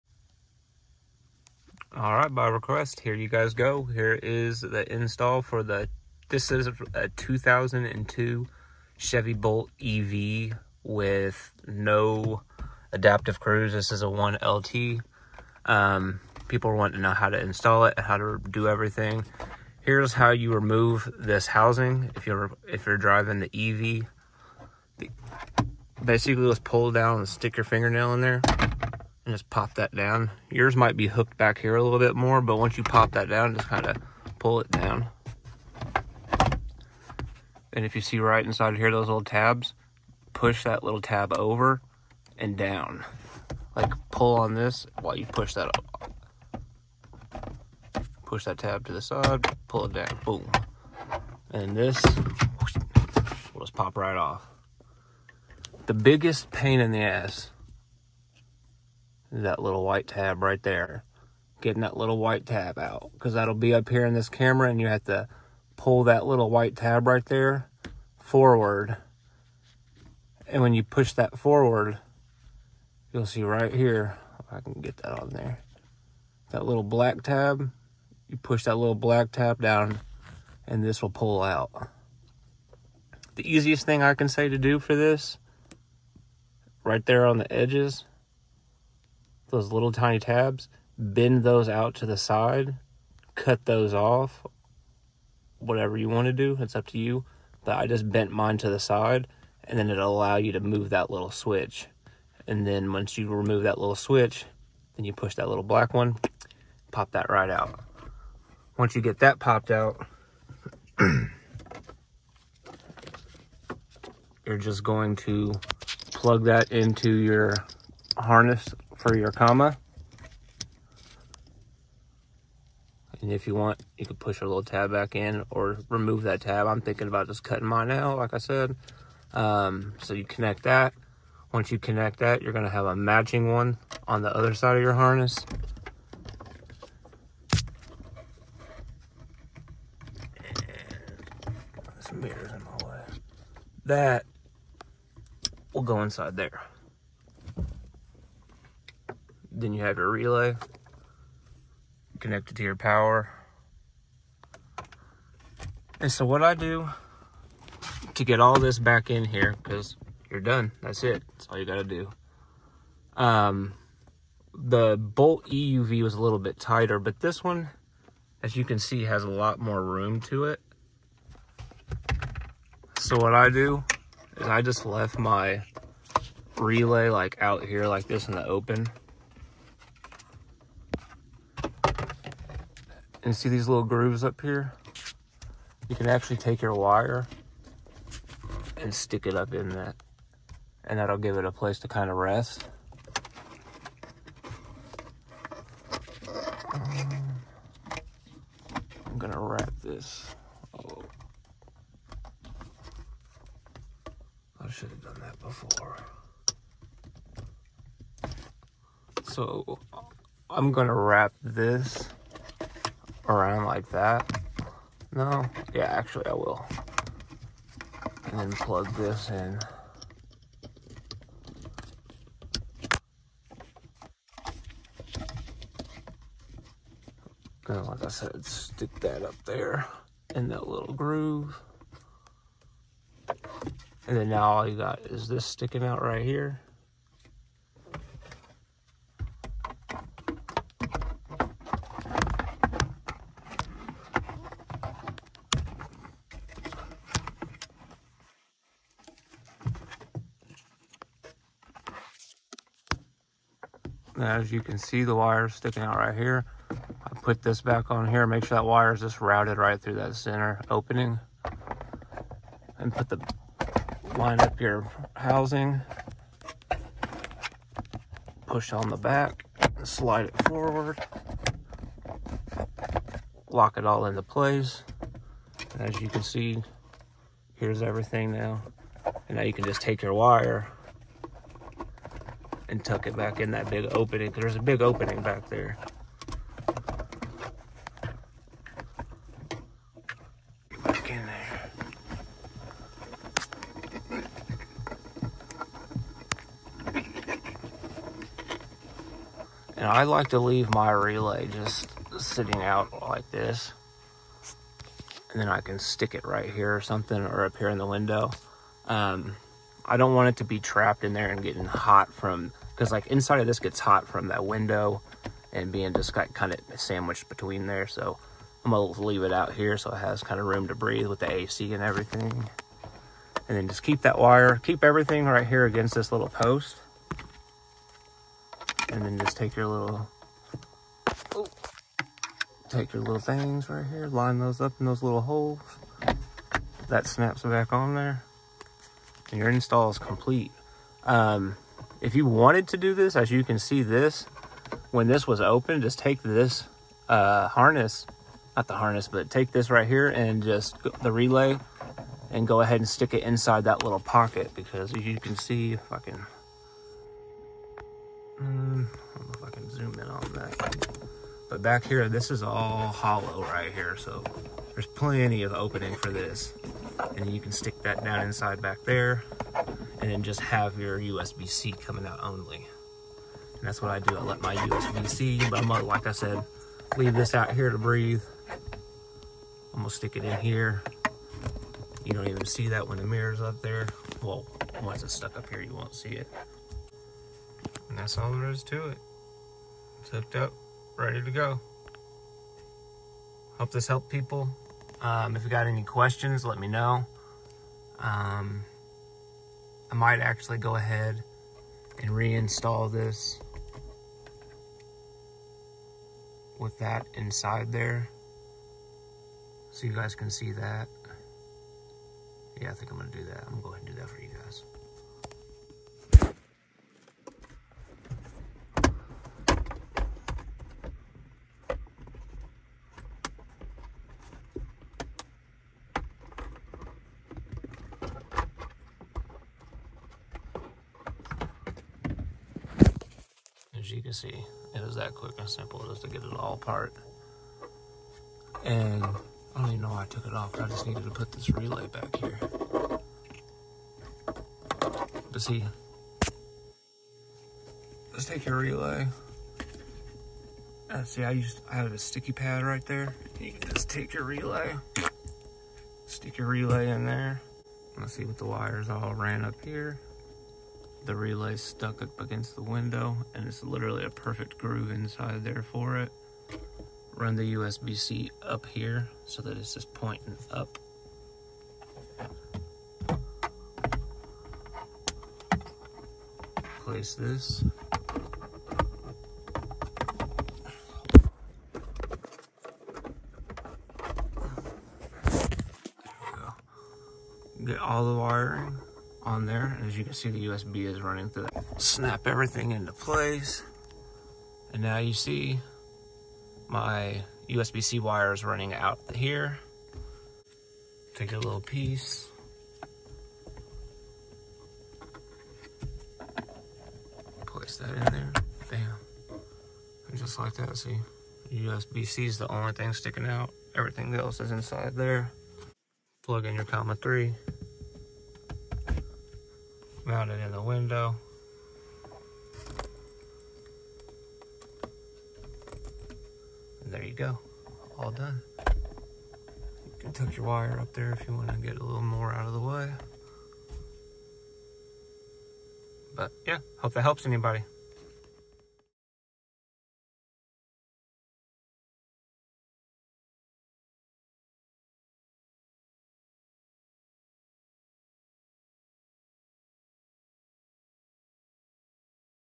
How to install a comma 3 on the 2022 Chevy Bolt EV 1LT
Here’s a video showing how to install a comma 3 into a Chevy bolt ev. Shows how to tuck the wires 2 different ways and the installer link is at the end of video. I’ll also link here in discription.

URL INSTALL LINK

Installer.comma.ai/twilsonco/sunny-Dev-no-acc-fault